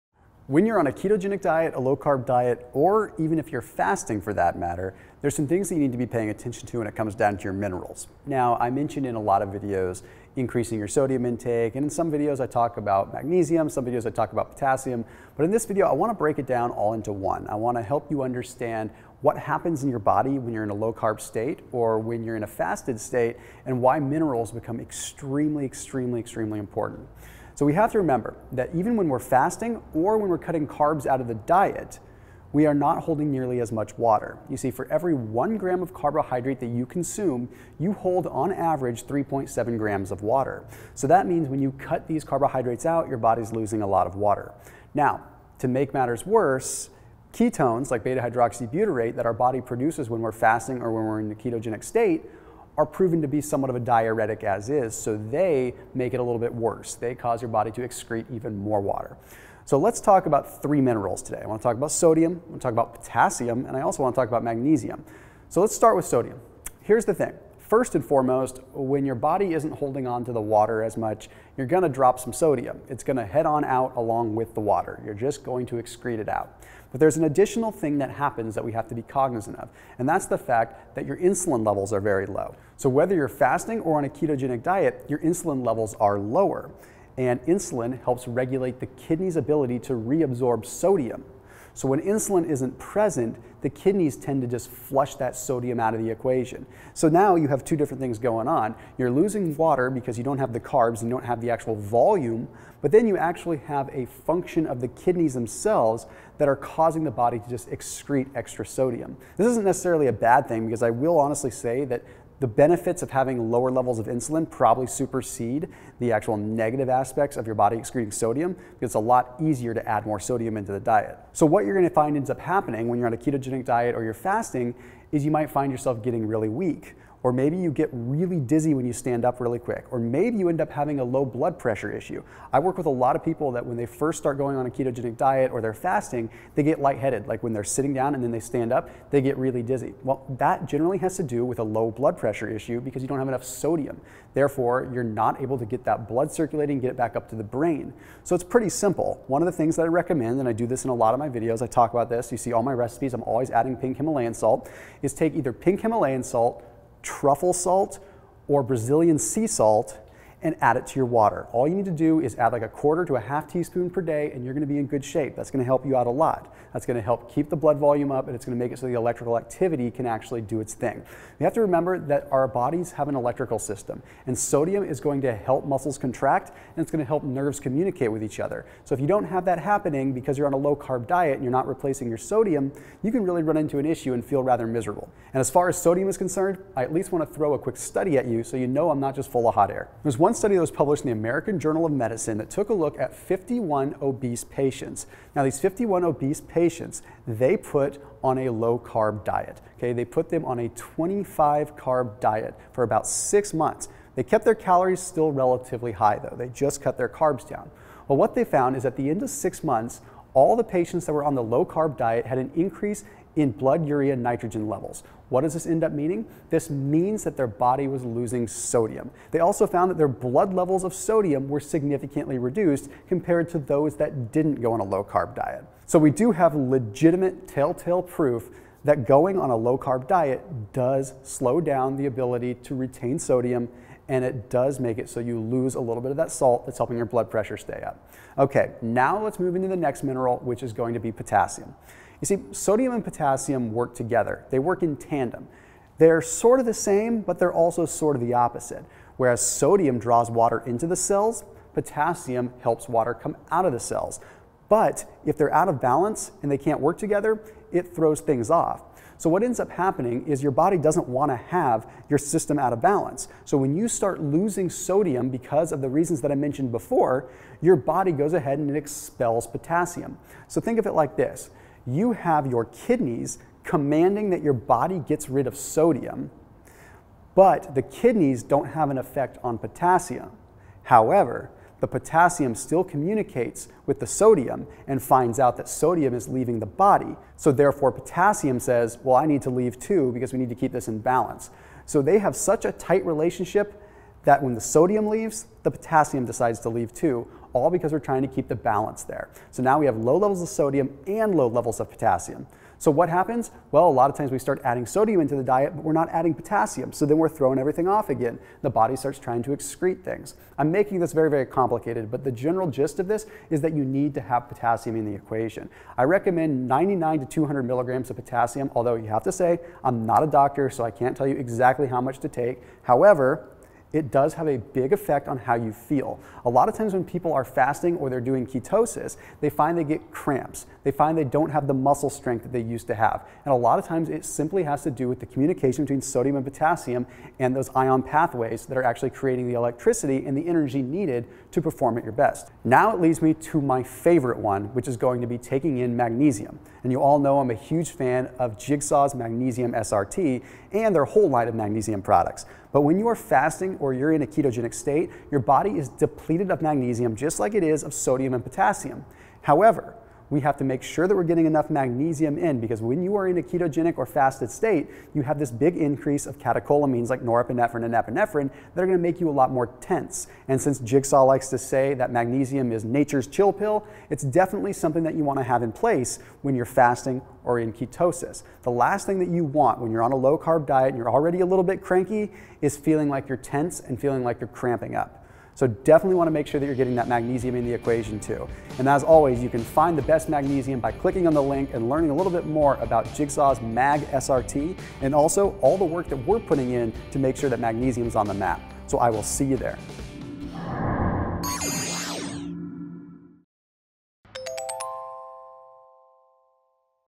Why Minerals are so Important in a Low Carb Diet
Trying a Low Carb Diet or Fasting?

Thomas DeLauer​ breaks down why minerals are so crucial during low carb diets and fasting on the latest

Make sure you are meeting the RDI 400mg of Magnesium per day with Jigsaw's MagSRT® – America's #1 Time-Release Magnesium Supplement